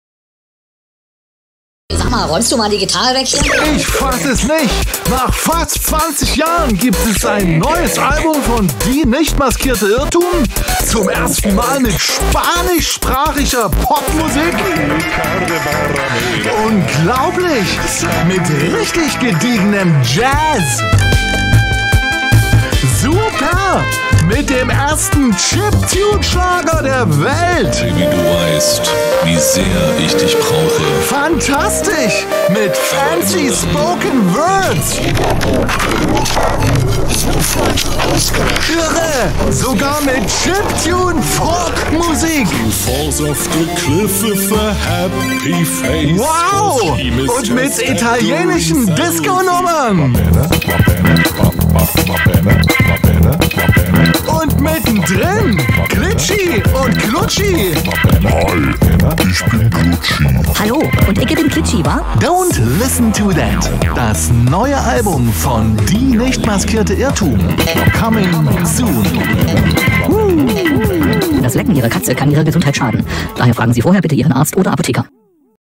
(don't listen to) That Happy Trailer!
That happy trailer! :) [audio only]
The release of the new die nmi! album is getting closer (probably in June/2021
Notice: This is the happy teaser. Still working on the "evil" teaser (full blown video!)... Cuz dark and bizarre stuff will happen at that album too.